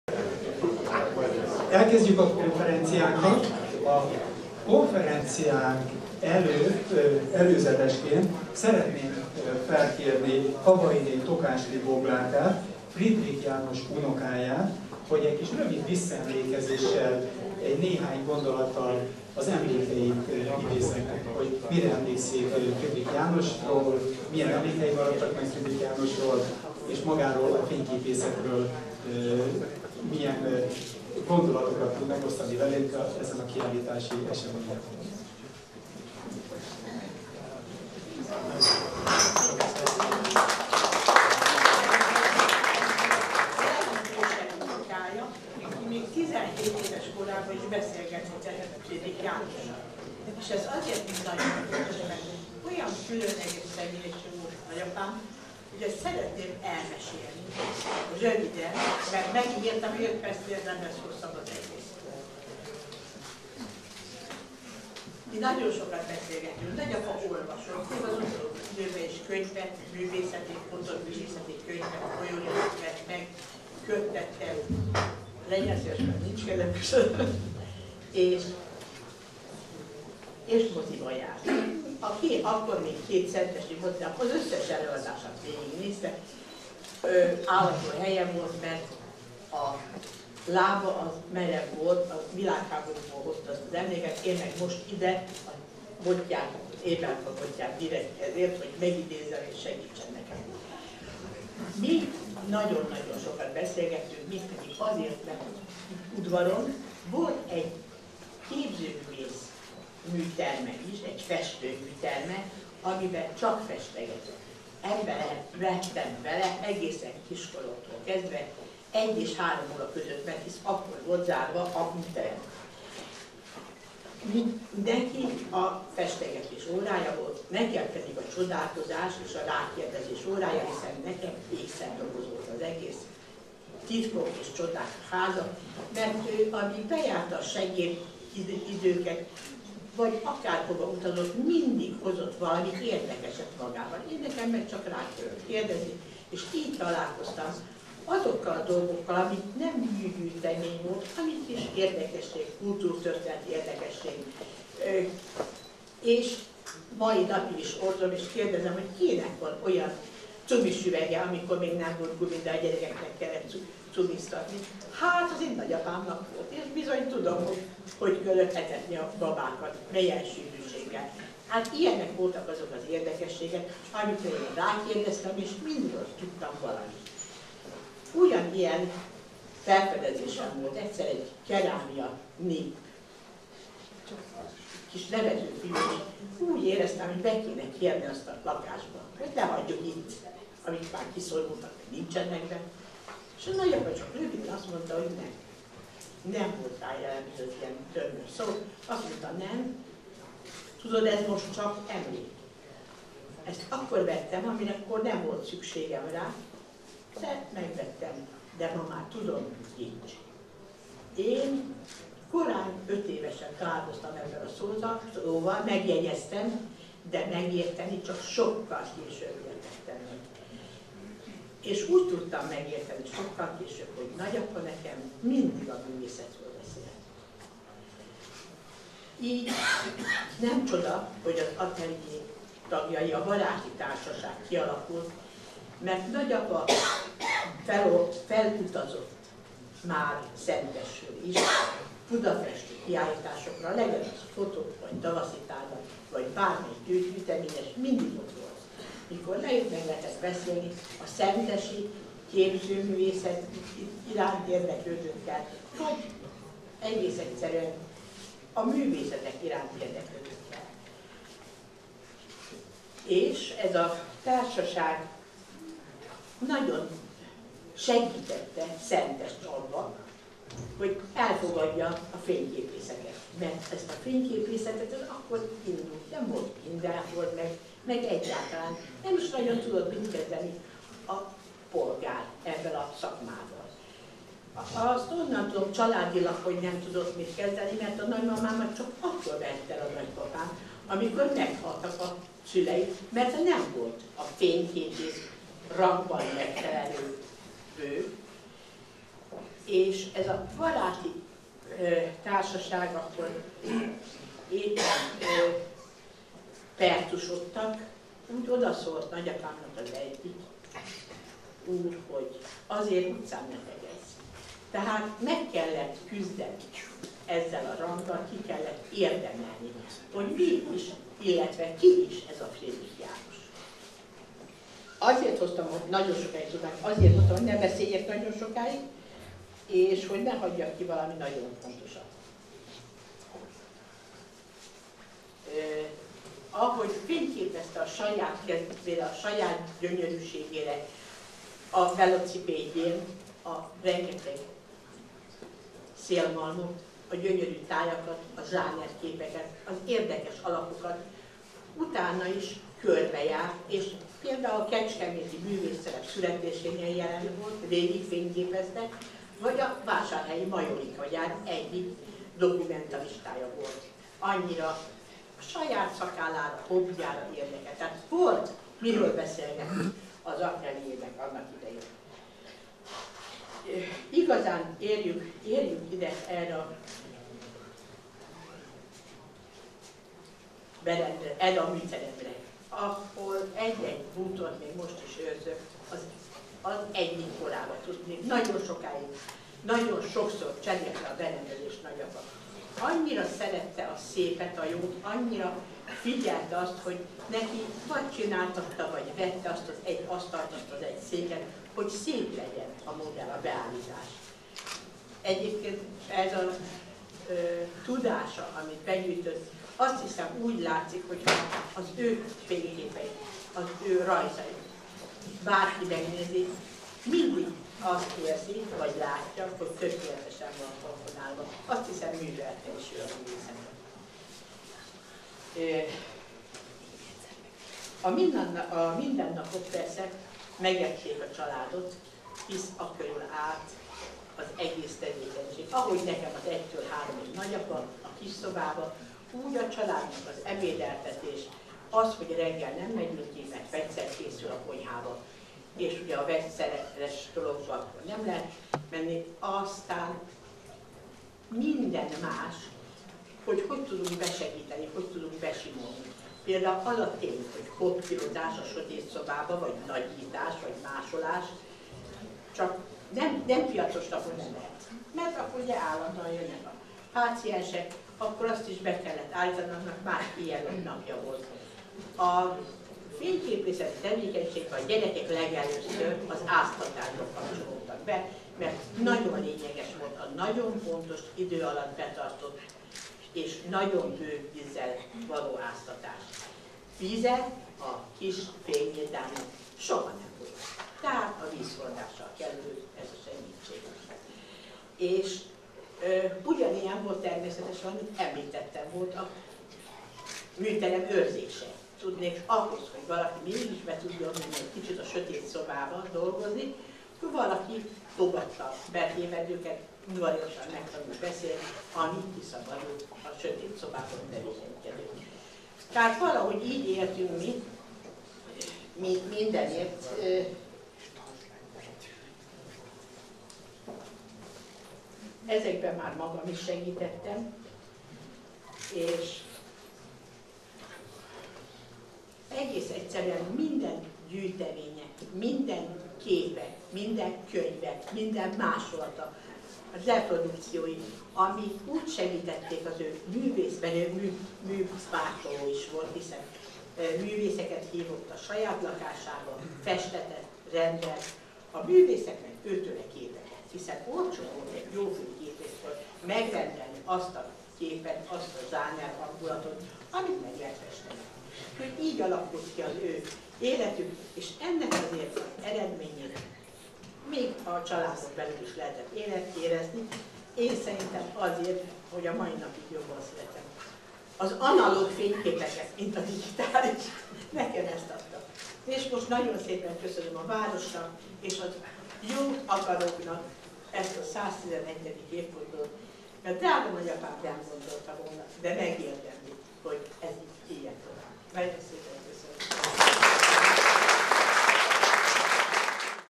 Kabainé Tokácsli Boglárka előadása
111 éves a Fridrich Fényírda Emlékkiállítás konferenciáján Kabainé Tokácsli Boglárka visszaemlékezése Fridrich Jánosról 2016-ban.